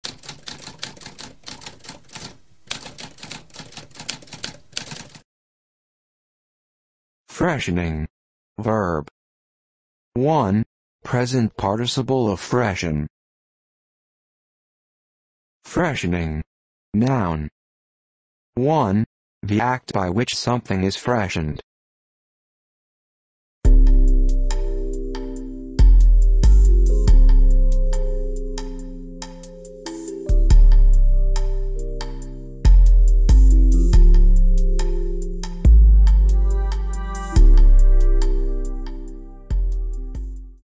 What does freshening mean?
A spoken definition of freshening.

Intro Sound:
Typewriter - Tamskp